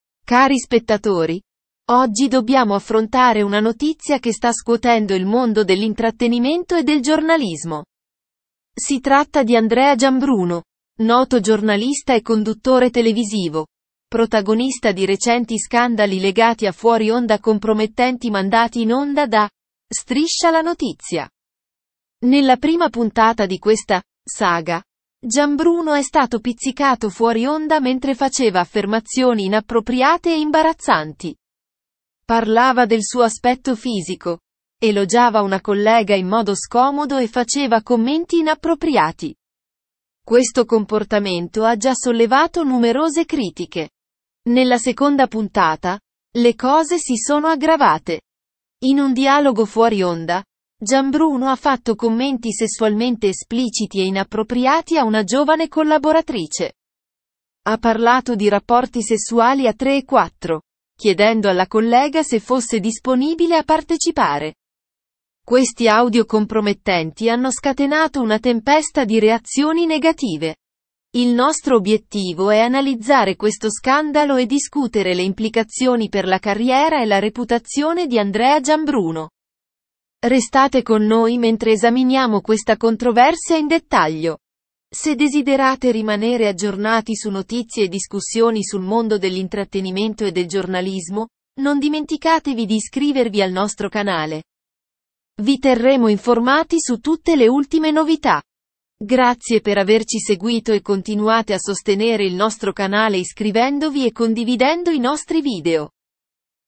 Scandalo Giambruno: Fuori Onda Compromettenti su Striscia la Notizia
Nel nostro video analizziamo il recente scandalo che coinvolge il giornalista e conduttore Andrea Giambruno, le cui affermazioni sessualmente esplicite sono state registrate in fuori onda e trasmesse da 'Striscia la Notizia'. Esaminiamo le implicazioni per la sua carriera e la discussione in corso.

Titoli brevi:
1. Giambruno: Fuori Onda Shock
2. Andrea Giambruno: Scandalo sessuale
3. Striscia la Notizia: Le registrazioni
4. Giambruno in crisi: Carriera a rischio